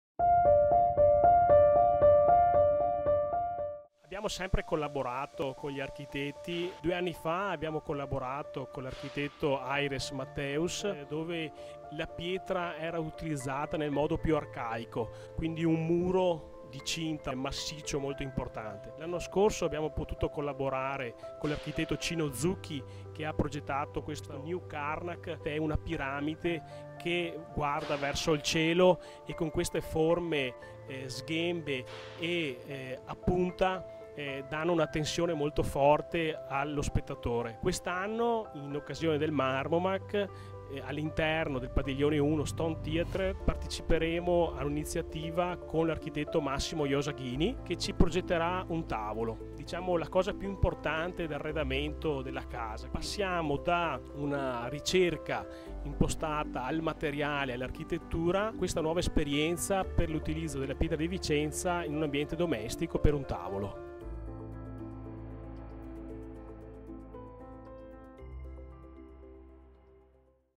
Marmomacc 2016: Massimo Iosa Ghini progetta un tavolo per Grassi Pietre
Abbiamo sempre collaborato con gli architetti: due anni fa abbiamo collaborato con l’architetto Aires Mateus dove la pietra era utilizzata nel modo più arcaico, quindi un muro di cinta, massiccio, molto importante.
L’anno scorso abbiamo potuto collaborare con l’architetto Cino Zucchi che ha progettato questo New Karnak che è una piramide che guarda verso il cielo e con queste forme sghembe e a punta danno una tensione molto forte allo spettatore.
Quest’anno in occasione del Marmomacc all’interno del Padiglione 1, Stone Theatre, parteciperemo a un’iniziativa con l’architetto Massimo Iosa Ghini che ci progetterà un tavolo, la cosa più importante d’arredamento della casa.
Passiamo da una ricerca impostata al materiale e all’architettura a questa nuova esperienza per l’utilizzo della Pietra di Vicenza in un ambiente domestico, per un tavolo.